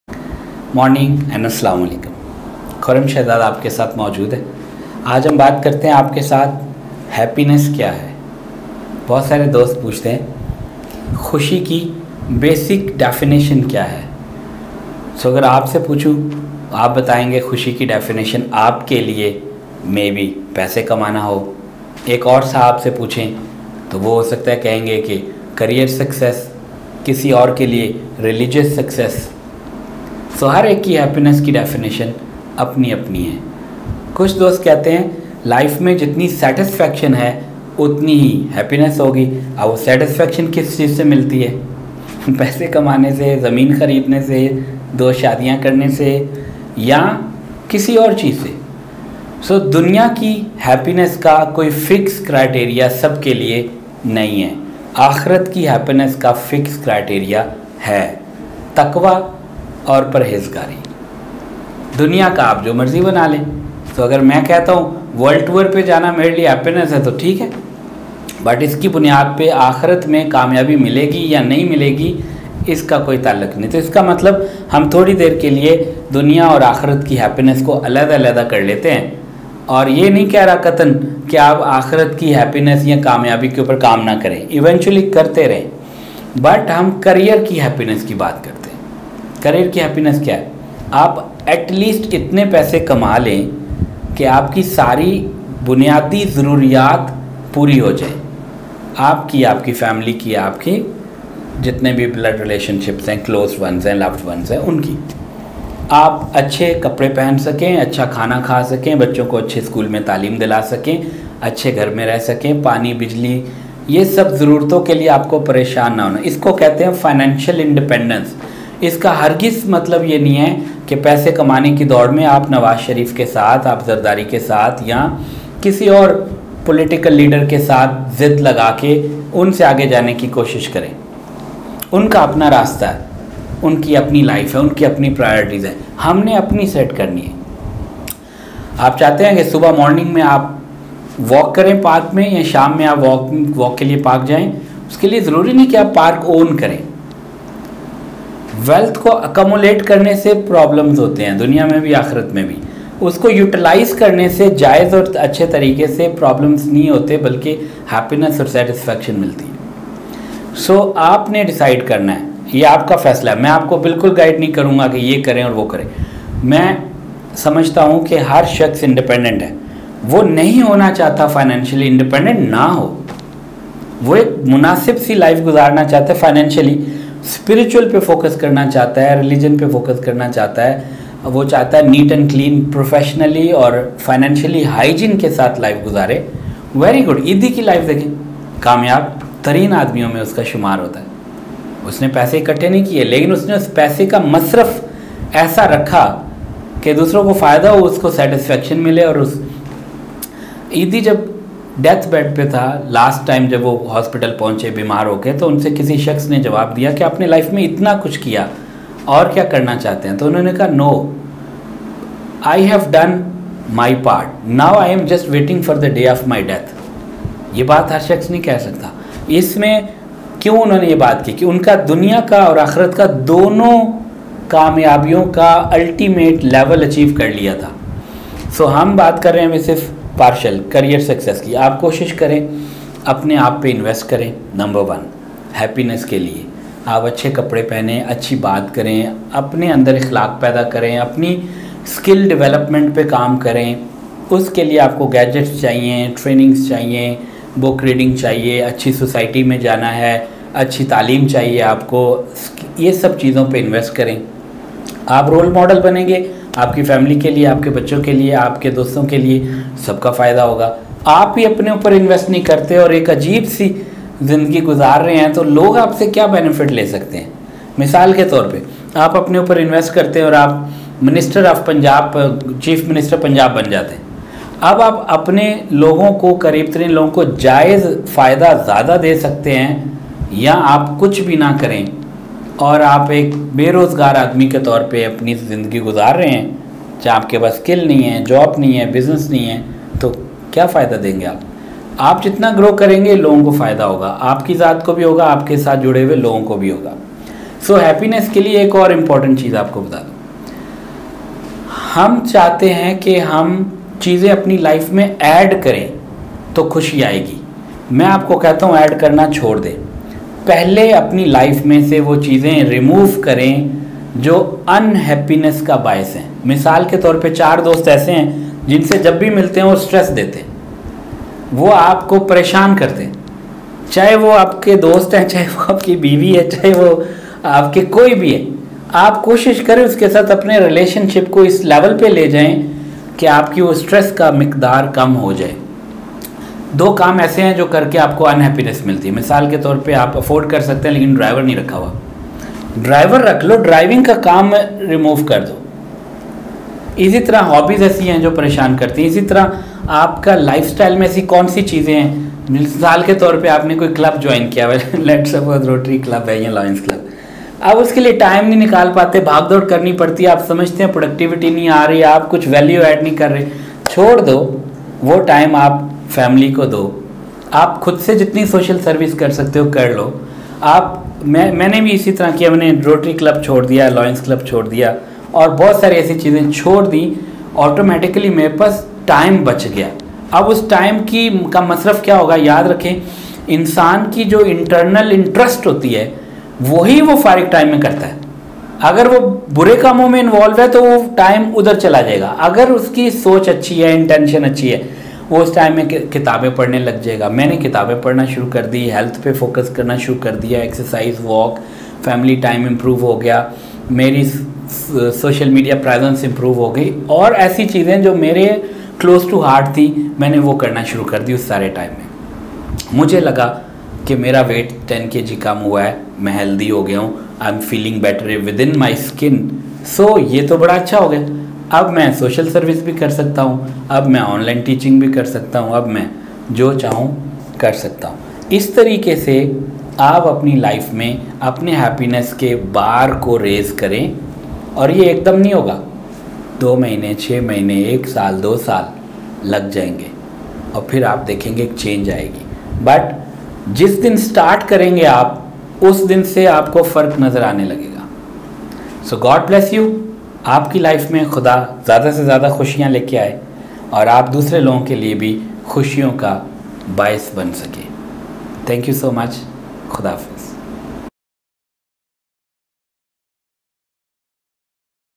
What is happiness - Learn the basic understanding
Khurram Shahzad is explaining the basic concept of happiness and professional success.